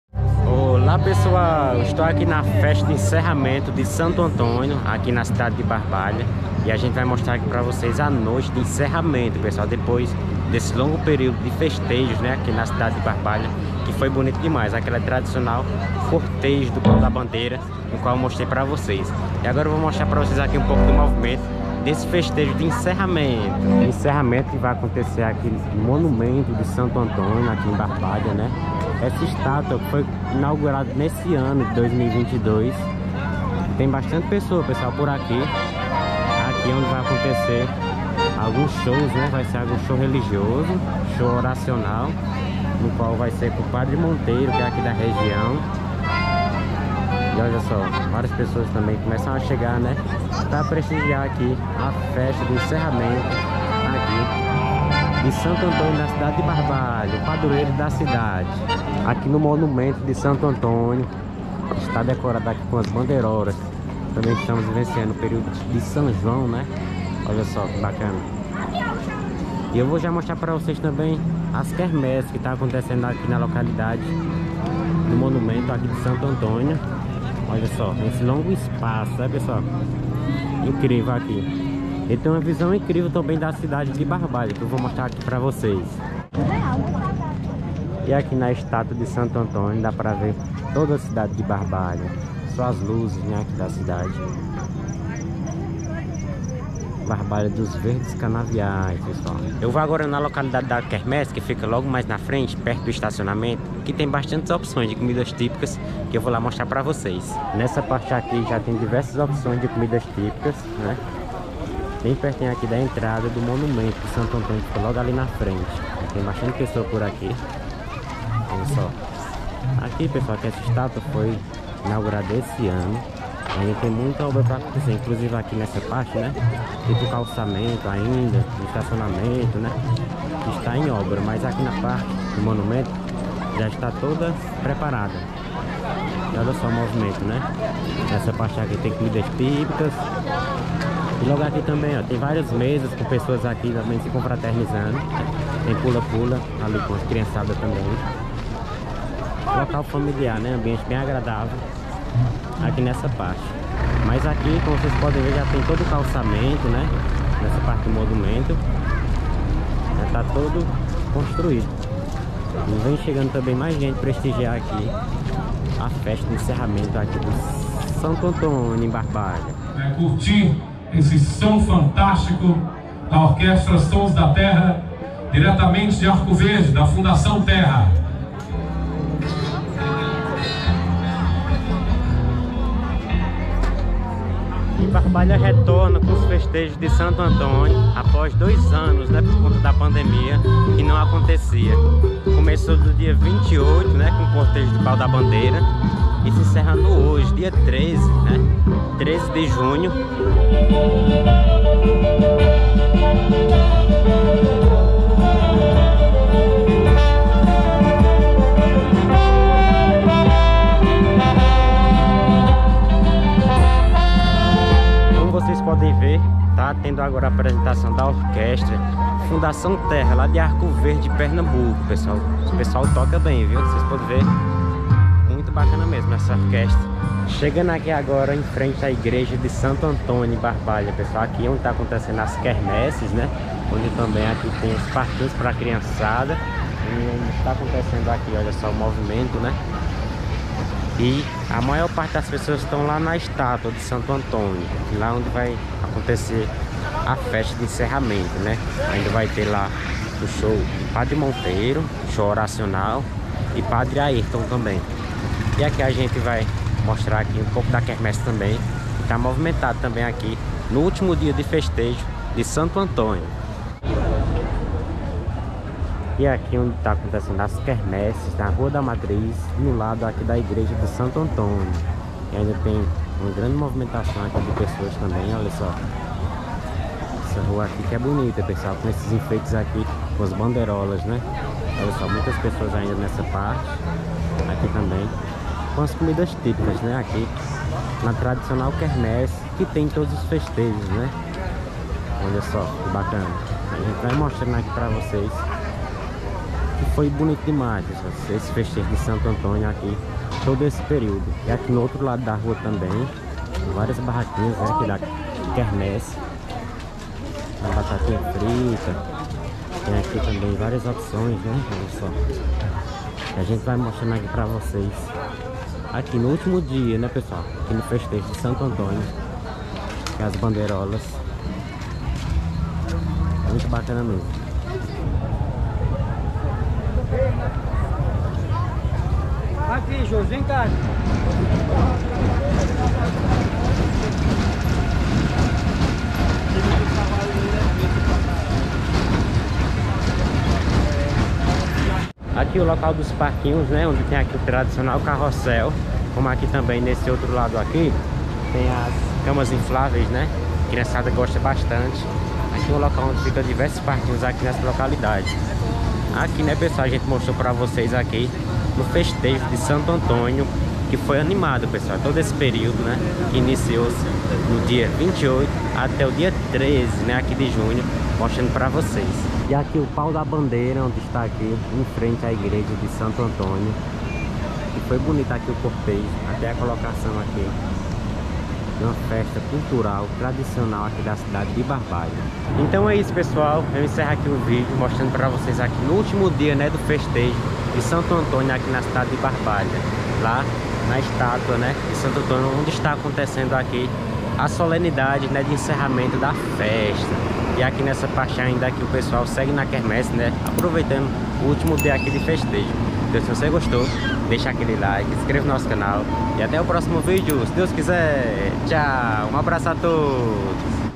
Último dia de Festejo de Santo Antônio em Barbalha-CE Muita Gente no Encerramento - 13/06/2022
Festa de Santo Antônio em Barbalha-CE.

Meu Instagram: @andredocariri